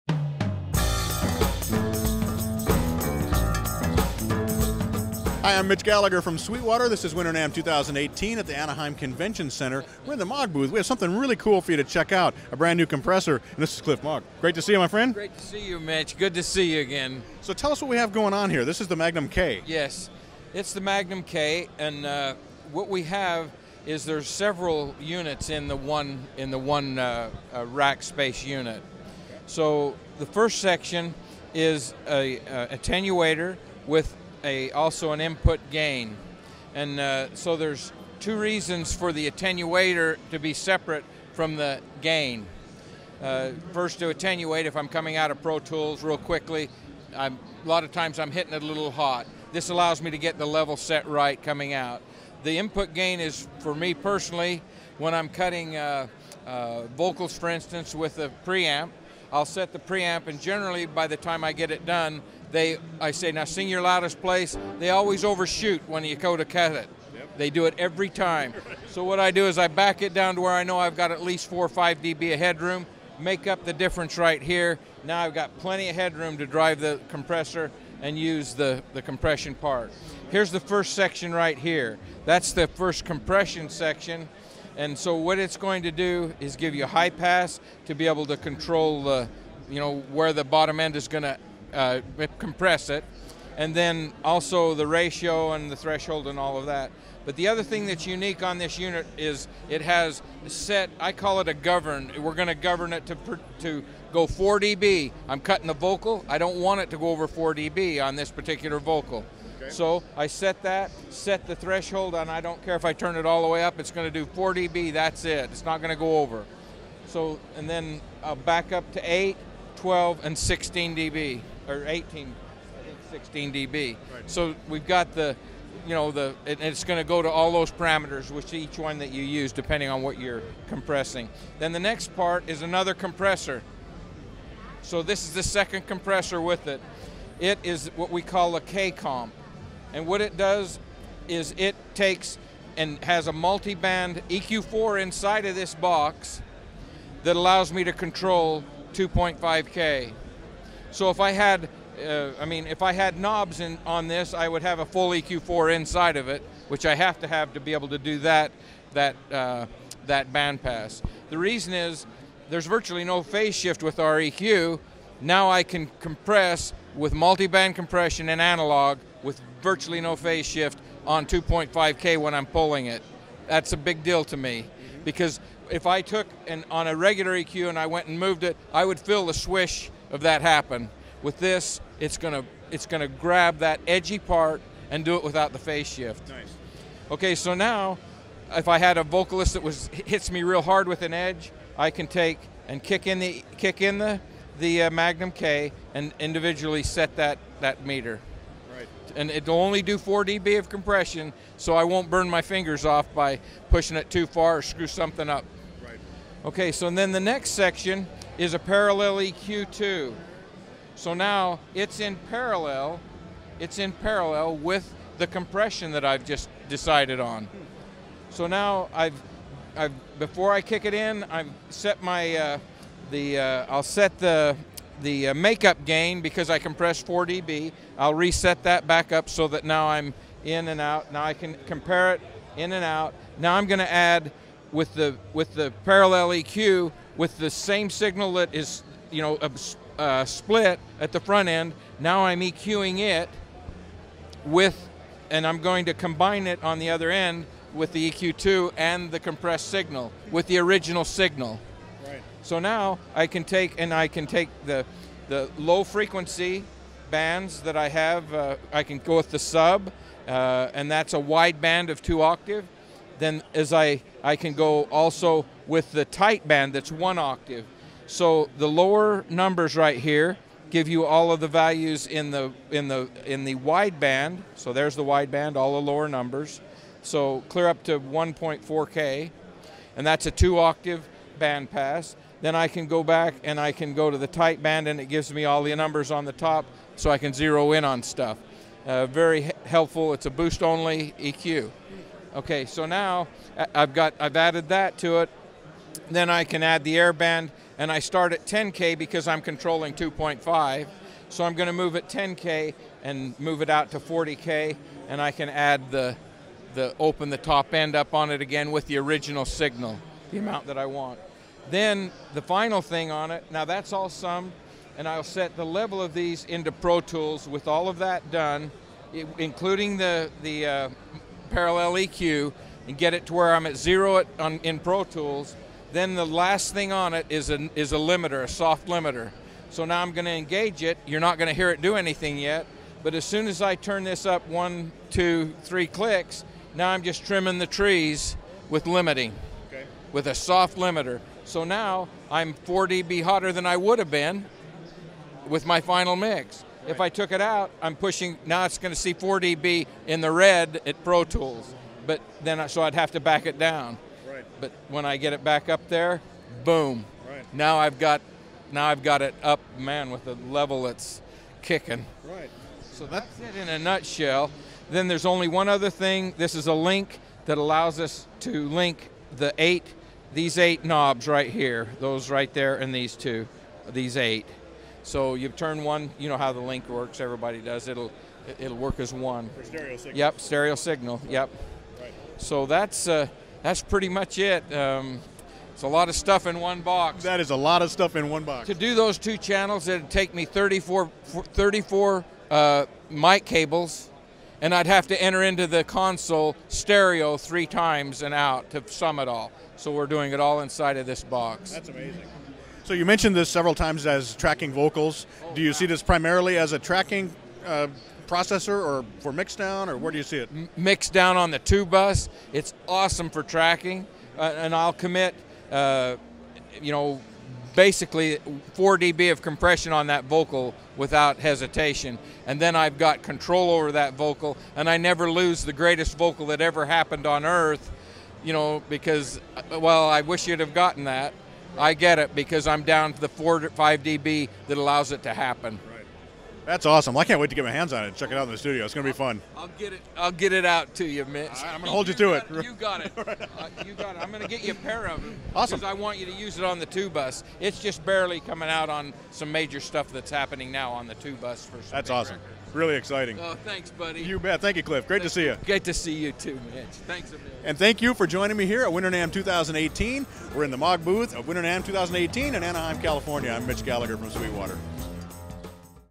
Maag Audio Magnum-K Compressor at Winter NAMM 2018
Mitch Gallagher heads to the Maag Audio booth at Winter NAMM 2018 to learn about the Magnum-K compressor.